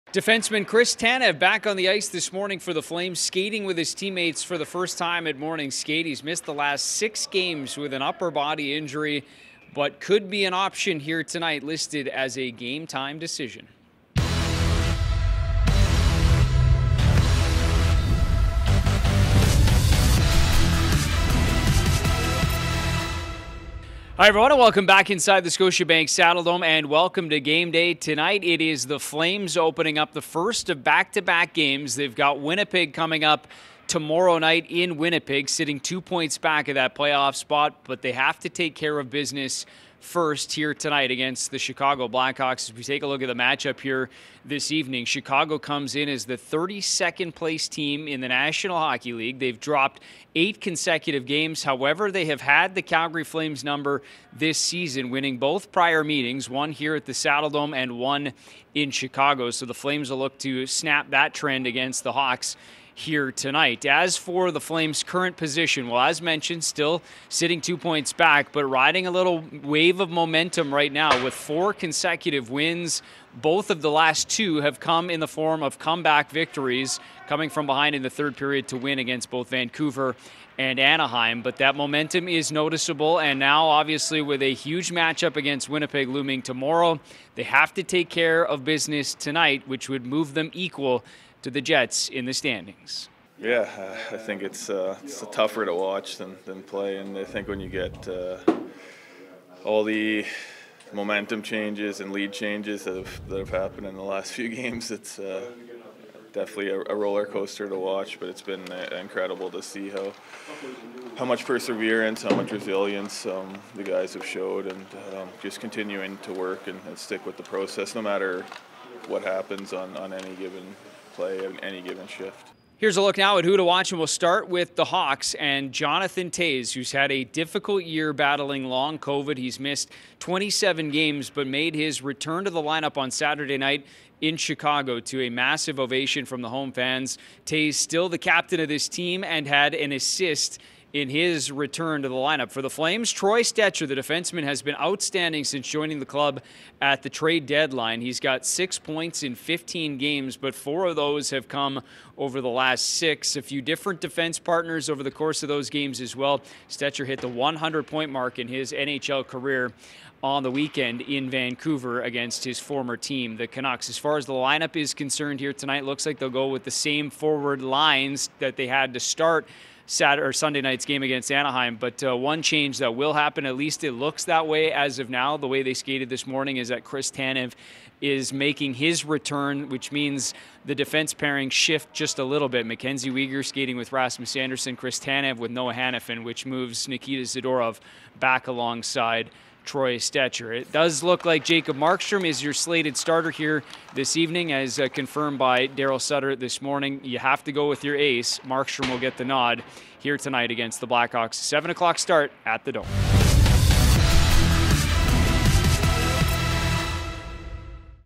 Game Day - 04.04.23
Brendan Parker previews tonight's game vs. the Blackhakws at the 'Dome.